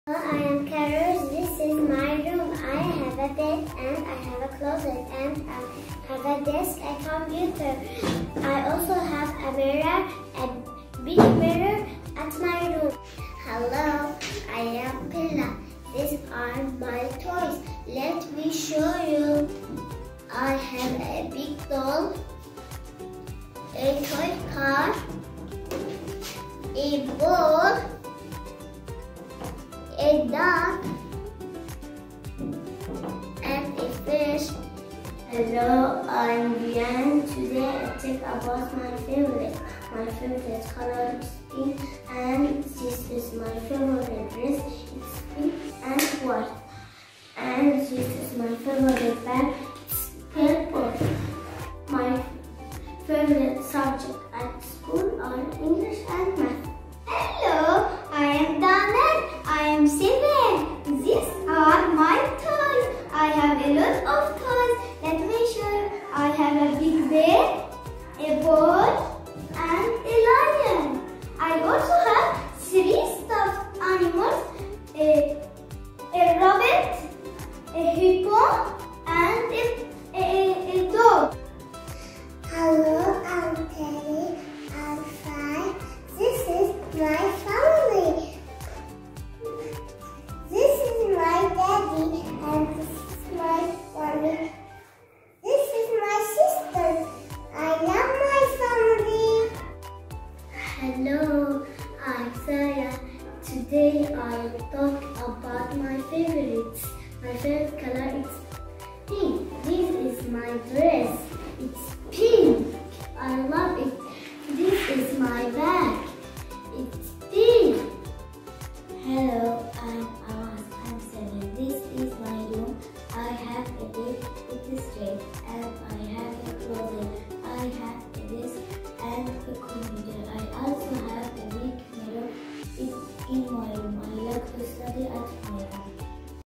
BKC Beeno Kids Describing their Rooms Toys Familyطلاب البينو يتحدثون عن العائلة و الألعاب و غرفهم
Beeno kids
Level 2️⃣

Bright Kids Center